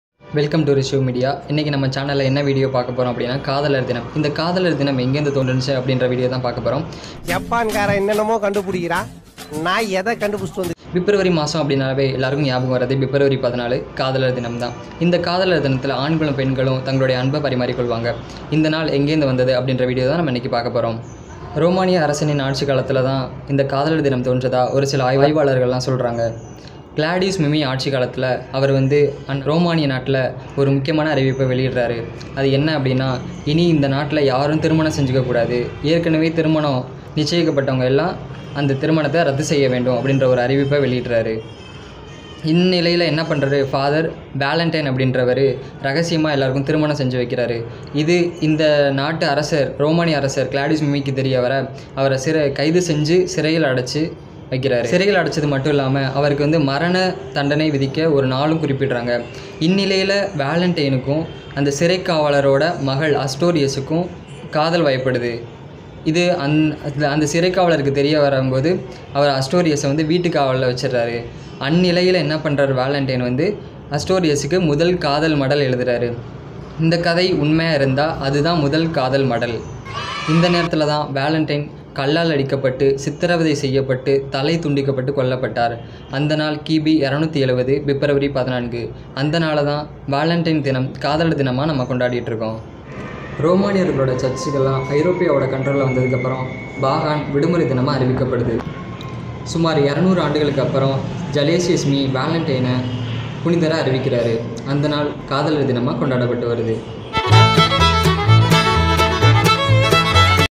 காதலர் தினம் முதன்முதலில் எப்படி உருவானது/First valentine's day came about in tamilstory/RISHO MEDIA
how valentine's day came about in tamil

interesting story for tamil

first lovers day in world

live Or let live